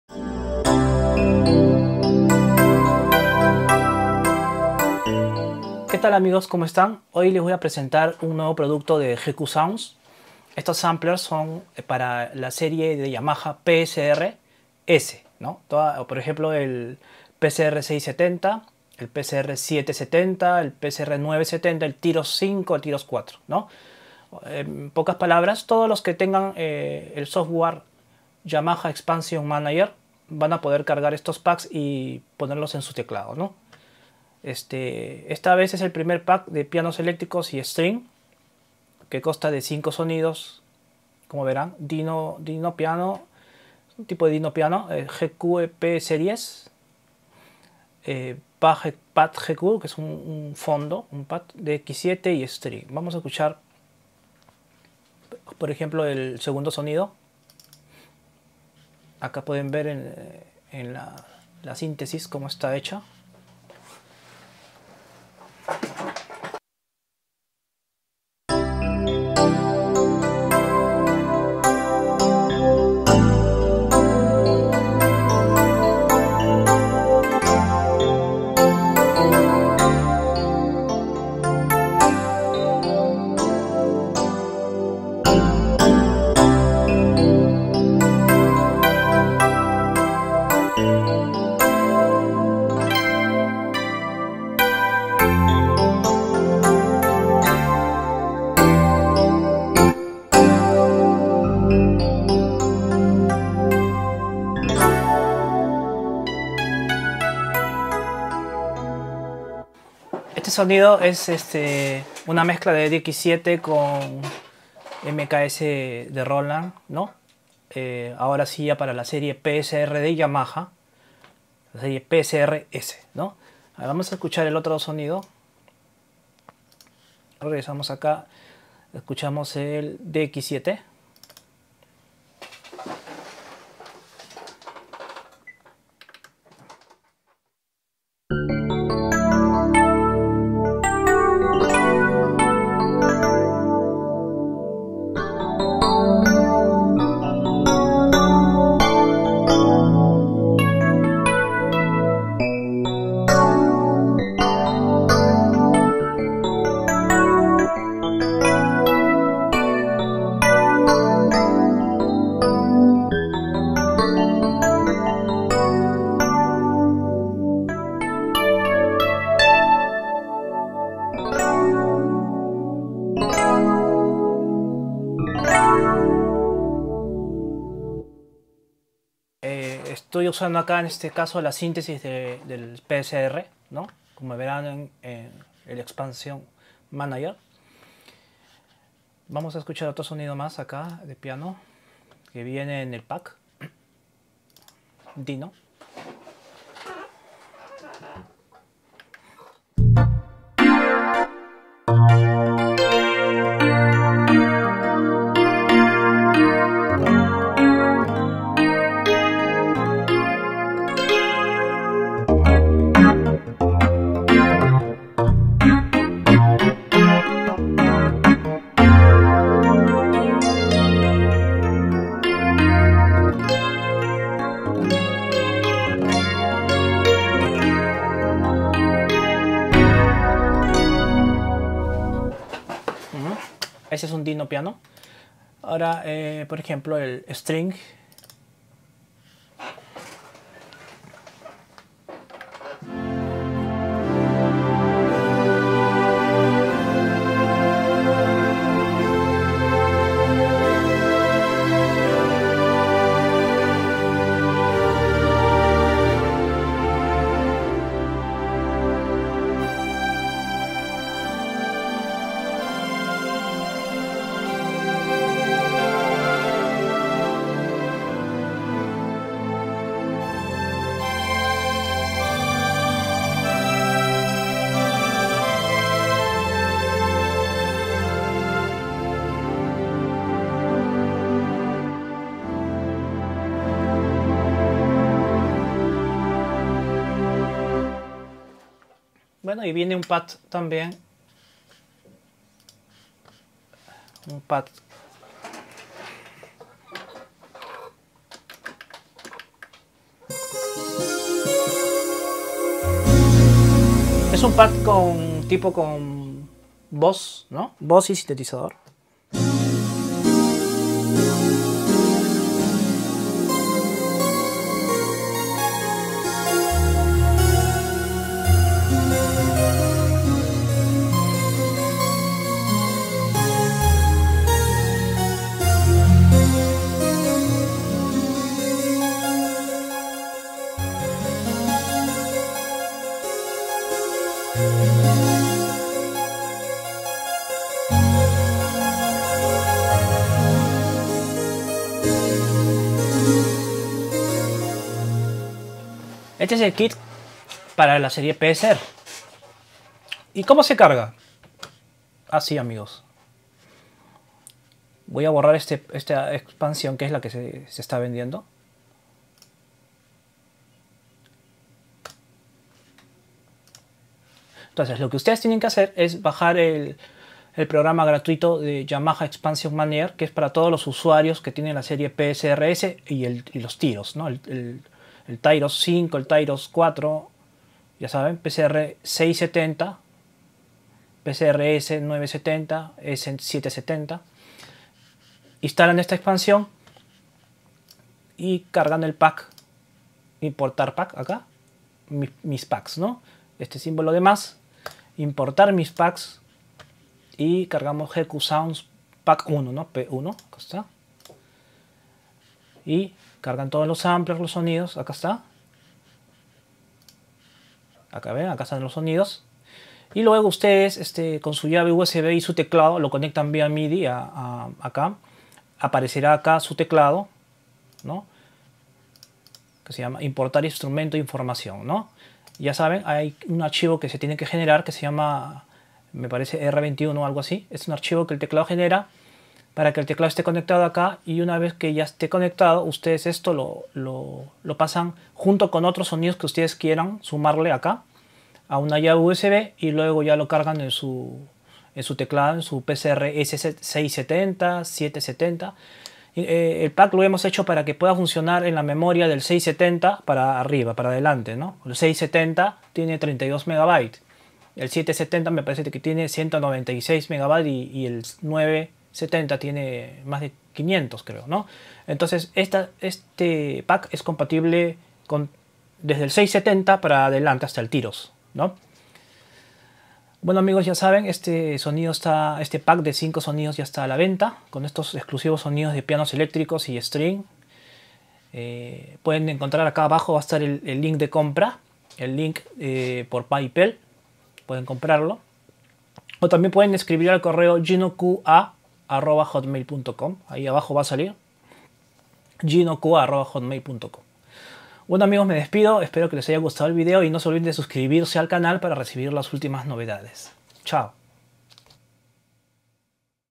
YAMAHA PSR S Genos, PSR SX, SERIES SAMPLES GQSOUND PACK1-2 compatible con expansion manager (YEM).
Pack de sonidos especialmente diseñados para la series psr genos y psr sx de yamaha usando el expansion manager de Epianos DX FM y string, tambien incluye piano acusticos, piano m1, y otros en la version 2. Solo carguelos en expansion manater YEM, y copie a su formato de teclado.
Musica compuesta e interpretada por gino quiñones ampuero.